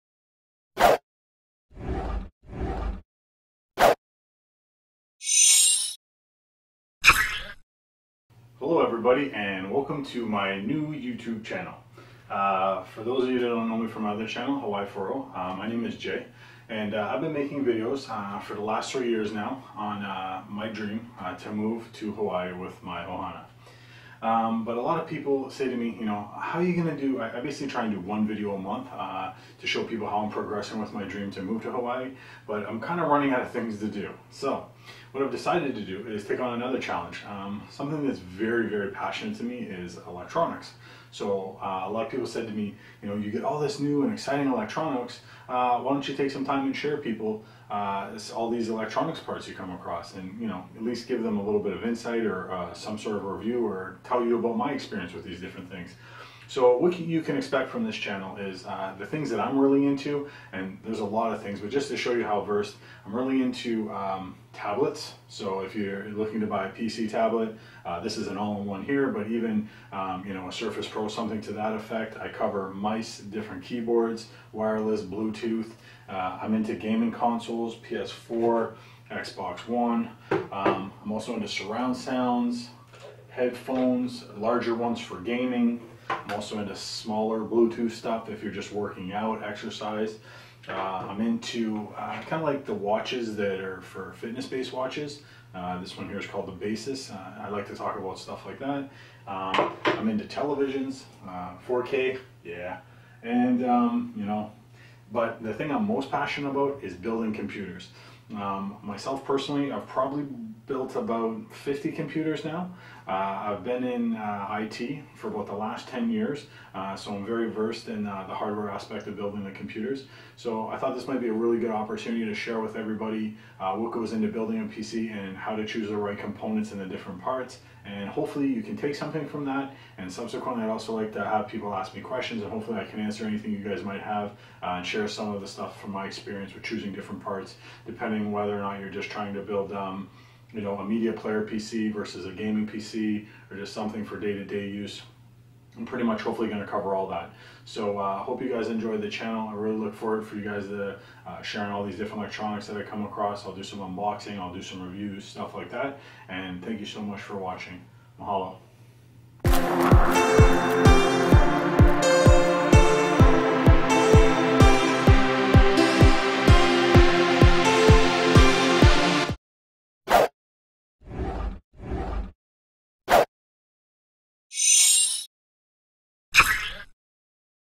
Intro
Hello This is the 1st Video for my new channel GeForce Jay :)

I will talk about the different products I plan to unbox in my upcoming videos